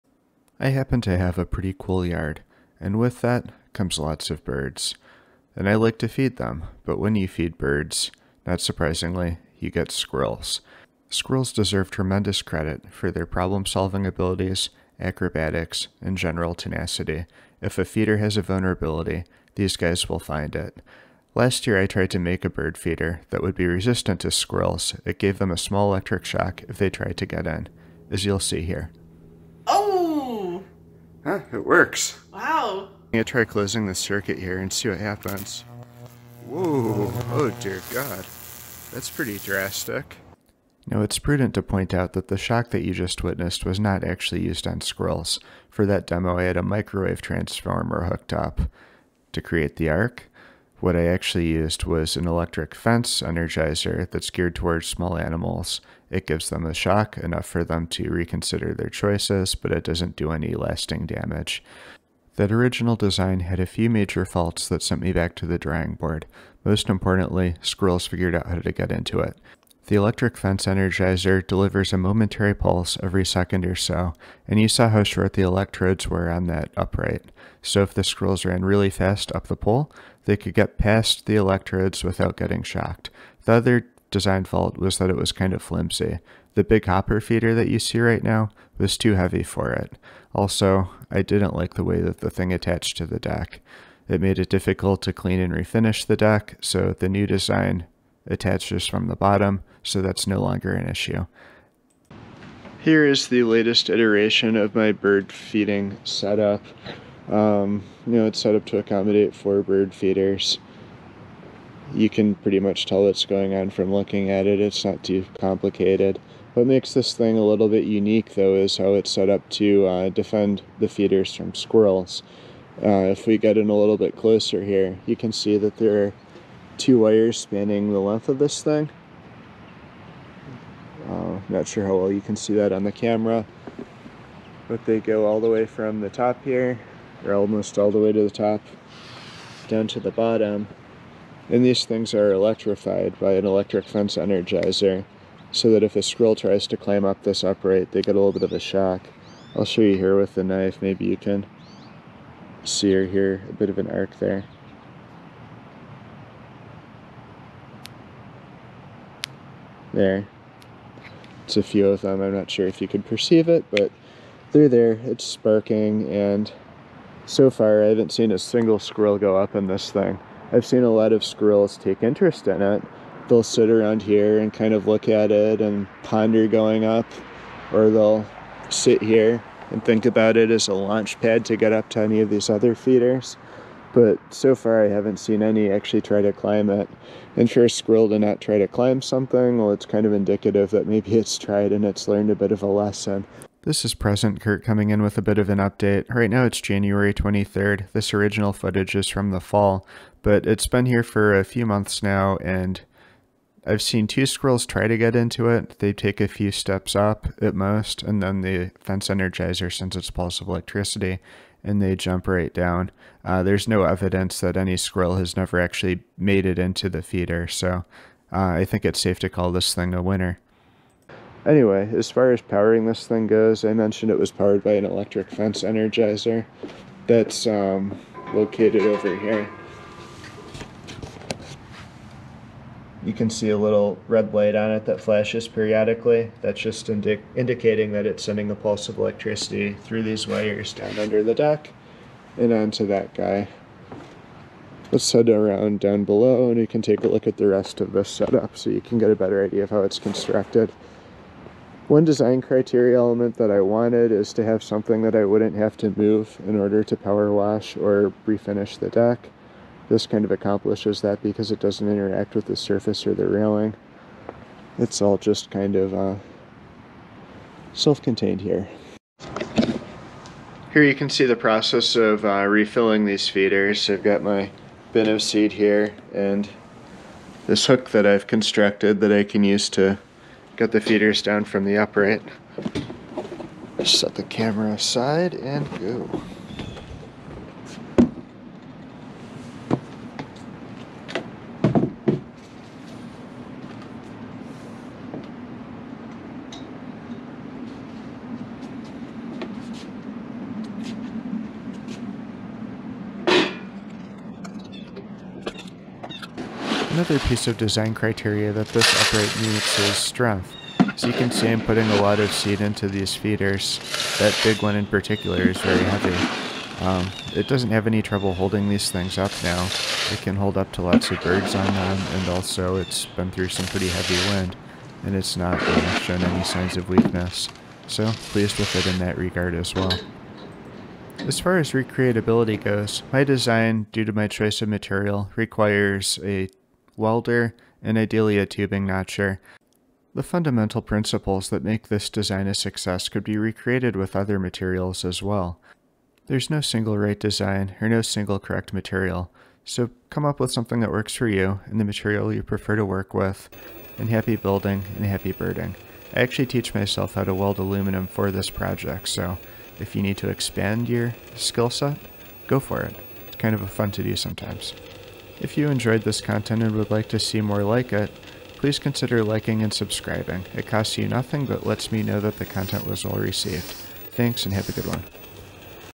Electronic Squirrel Defense 2.0 - How to keep squirrels out of your bird feeders!
This is v2.0 of my squirrel proof bird feeder design.  A link to the video on the original design is below.  This uses an electric fence energizer to make squirrels quickly reconsider their actions when they attempt to gain access to the bird feeders.  This has been in service for several months now and there's no evidence of a squirrel ever reaching a feeder.